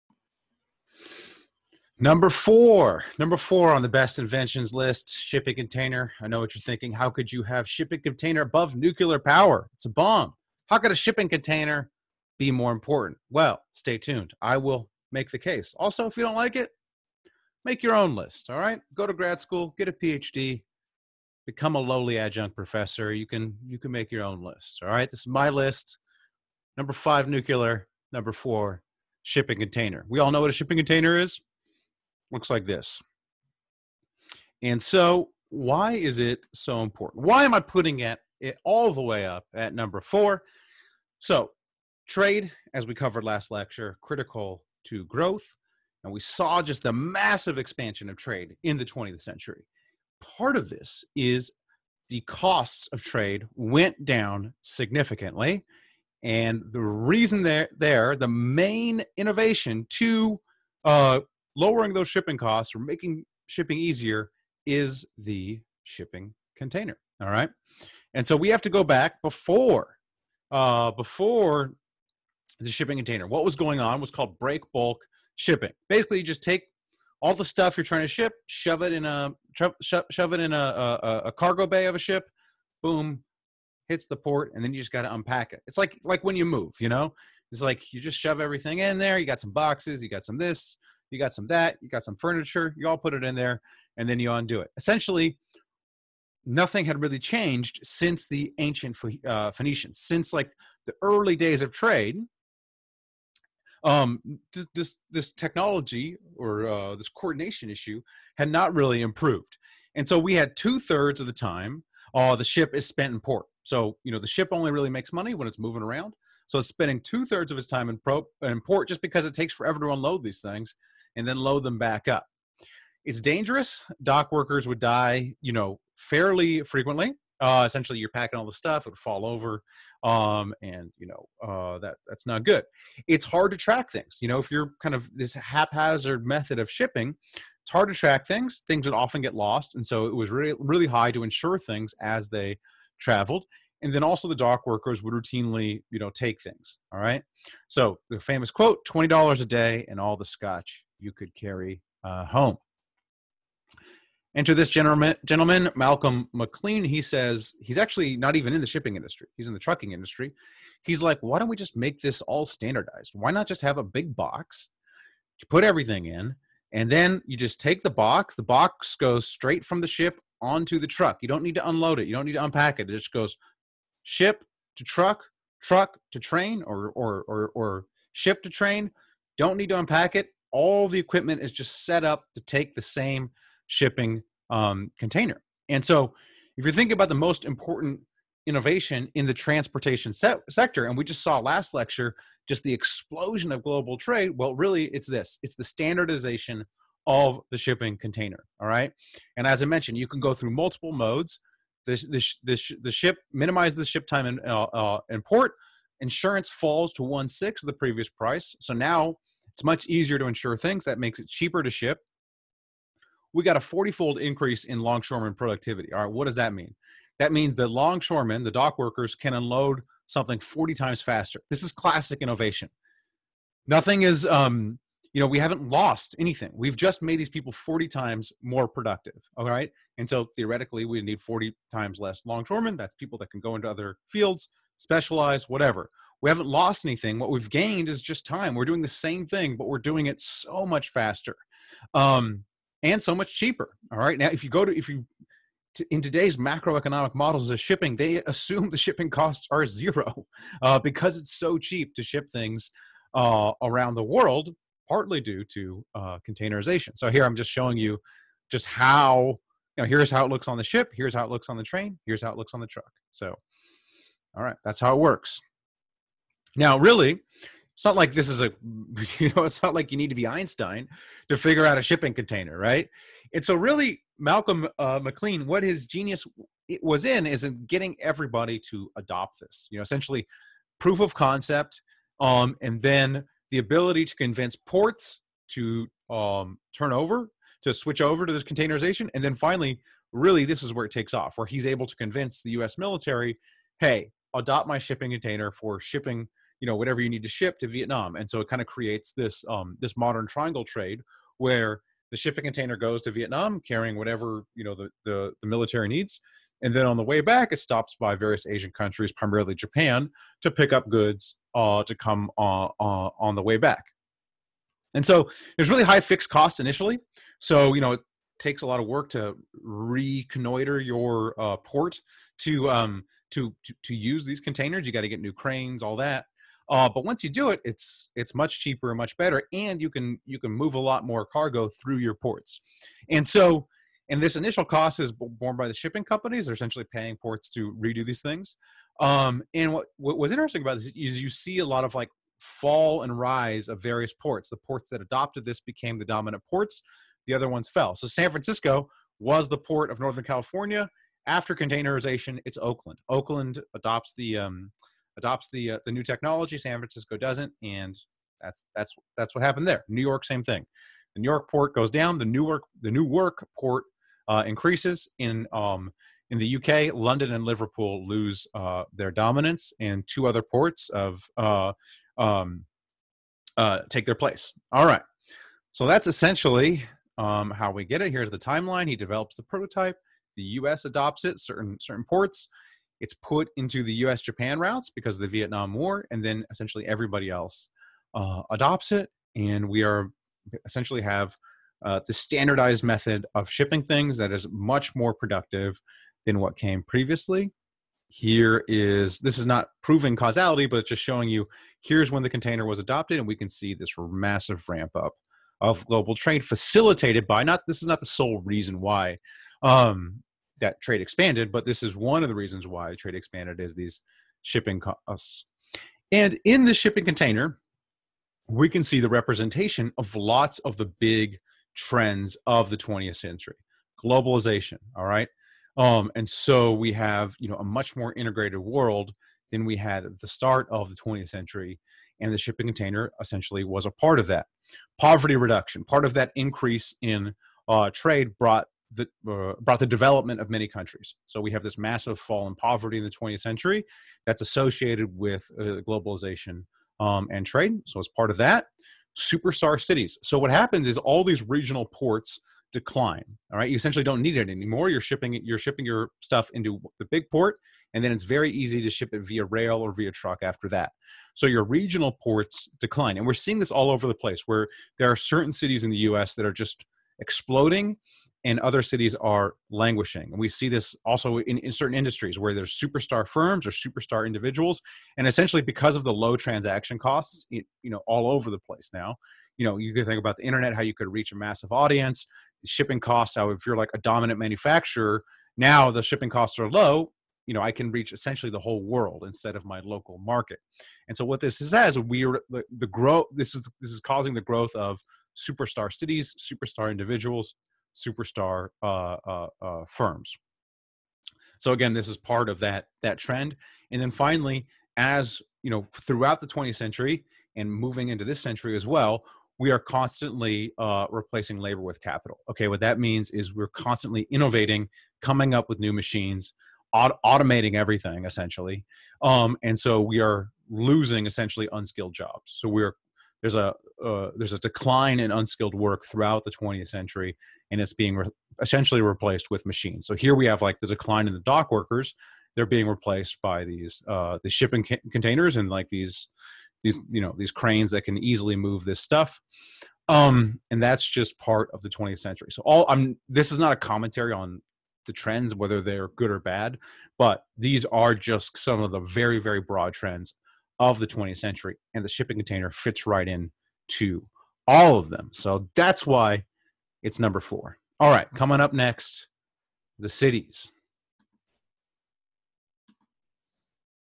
US Economic History: Inventions (Shipping Containers)
Part of the Inventions lecture series from my US Economic History class, taught in 2021.